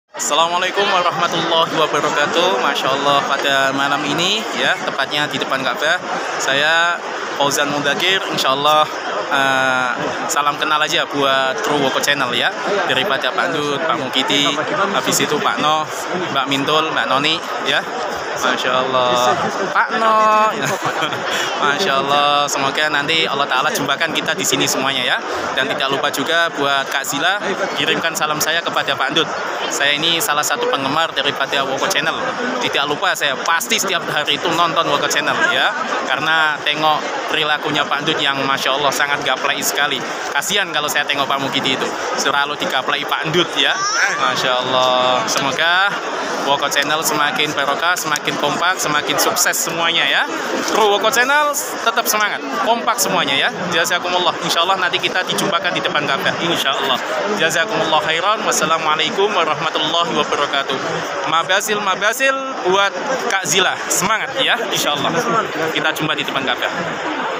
ucapan dan doa ustadz Fauzan di baitullah untuk pak Ndut woko channel dkk dan buat Zilla Mescha
Video dari ustadz Fauzan di Makkah Saudi Arabia